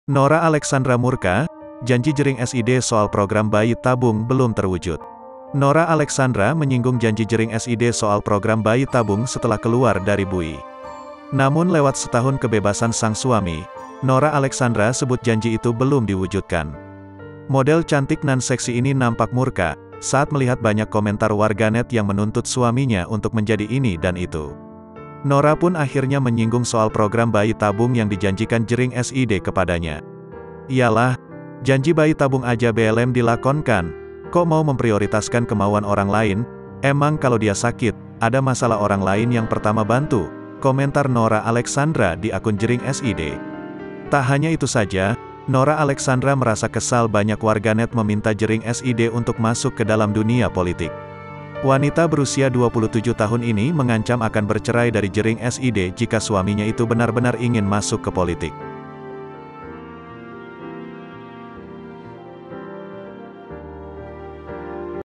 Nora Alexandra Murka, Janji Jerinx SID Soal Bayi Tabung Belum Terwujud
Nora Alexandra menyinggung janji Jerinx SID soal program bayi tabung setelah keluar dari bui.
Namun lewat setahun kebebasan sang suami, Nora Alexandra sebut janji itu belum diwujudkan.
Model cantik nan seksi ini nampak murka, saat melihat banyak komentar warganet yang menuntut suaminya untuk menjadi ini dan itu.
Nora pun akhirnya menyinggung soal program bayi tabung yang dijanjikan Jerinx SID kepadanya.